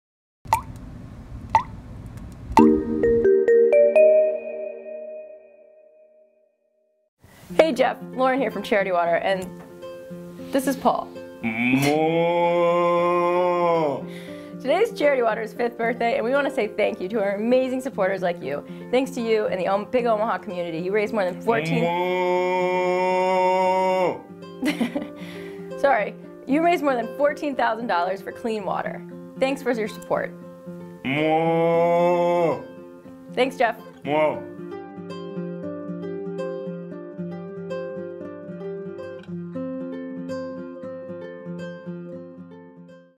Thank you, Jeff + Big Omaha community.
charity: water staff made this video for Jeff, to thank him and the the Big Omaha community for raising awareness and money for clean drinking water.

Jeff, it's our fifth birthday. But the celebration isn't about us -- it's about you. You've joined our mission to end the water crisis. You supported our cause. You've inspired us.